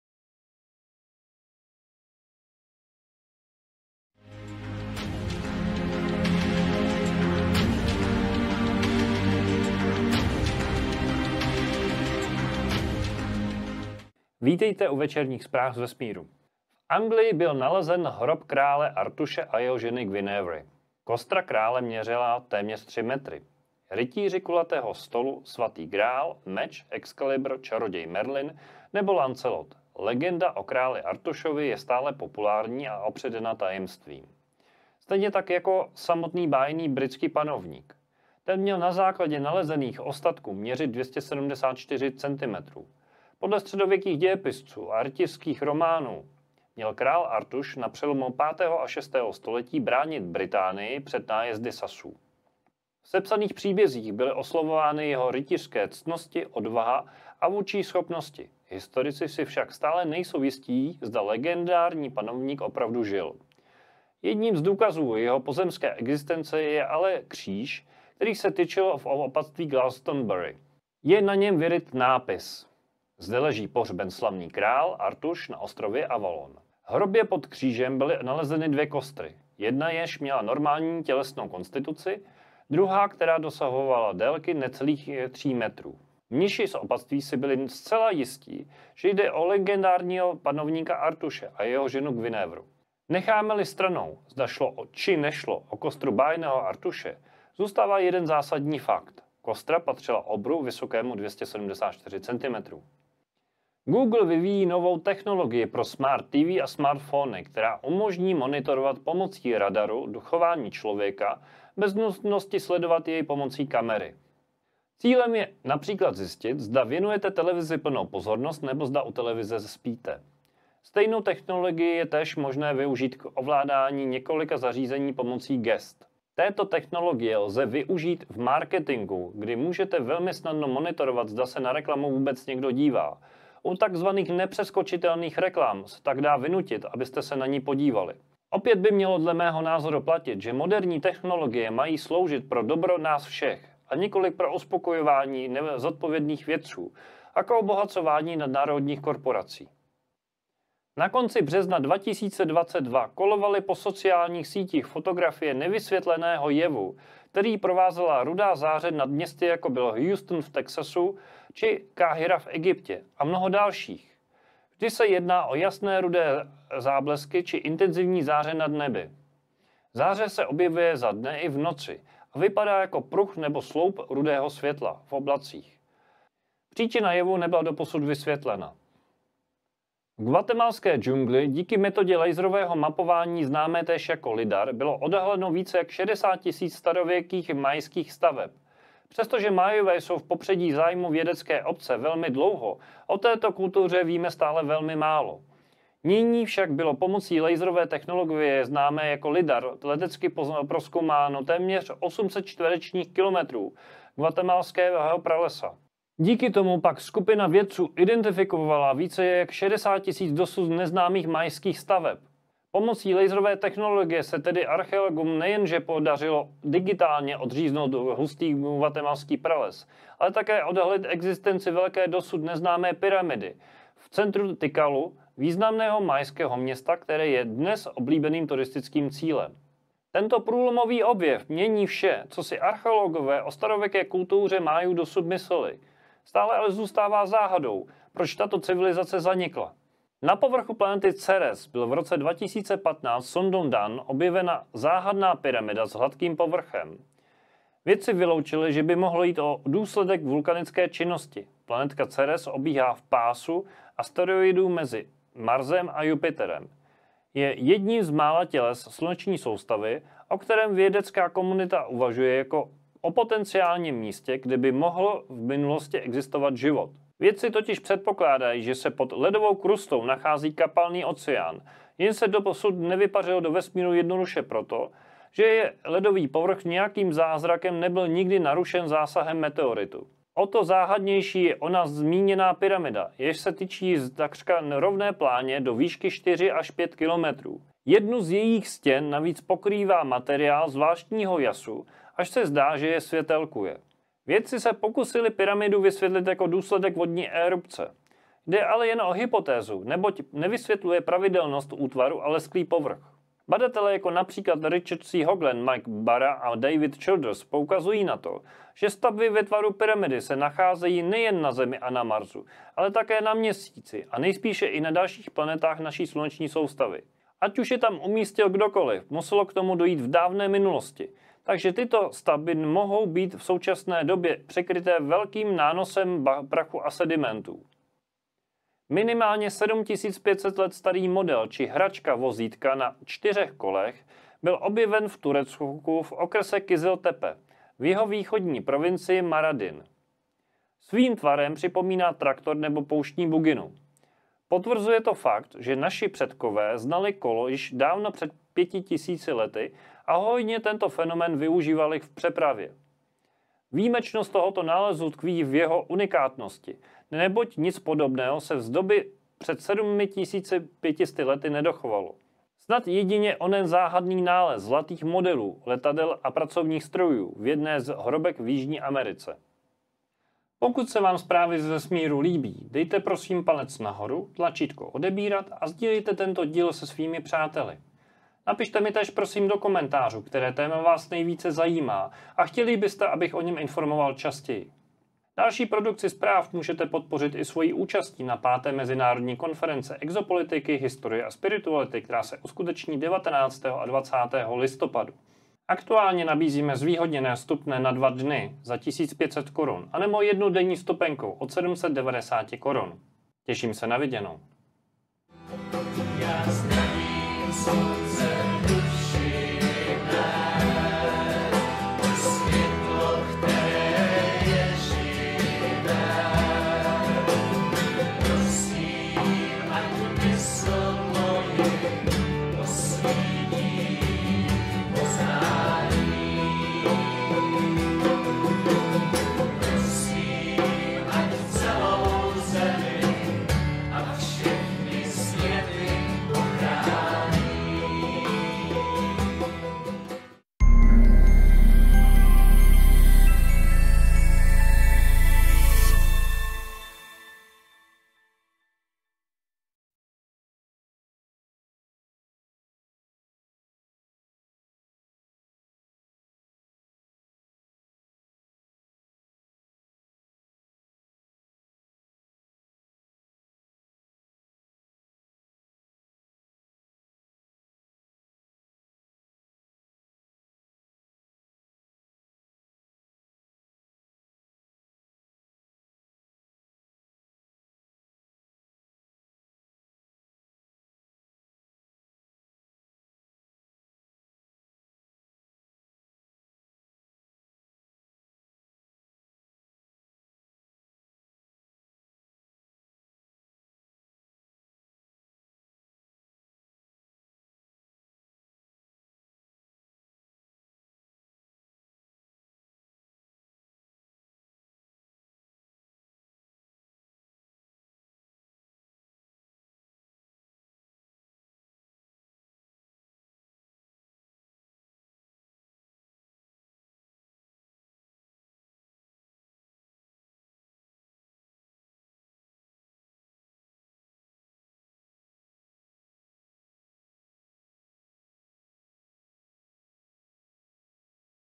V Anglii našli kostru obra (Zprávy z vesmíru #004)
Obsah:
00:00:00 Úvod
00:00:16 Obří kostra krále Artuše
00:01:43 Nová technologie monitoruje vaši aktivitu u smart zařízení
00:02:30 Červené záře nad městy
00:02:59 Fenomenální objev Mayského města
00:03:59 Pyramida na planetce Ceres
00:05:28 Auto z doby kamenné
00:06:09 Libí se vám zprávy?
00:06:44 Mantrovníci

Pokud se vám zprávy libí, dejte videu prosím LIKE a napište do komentářů, co by vás v takových zprávách nejvíce zajímalo! :-)

Vstupenky do sálu na konferenci od 790 Kč v prodeji na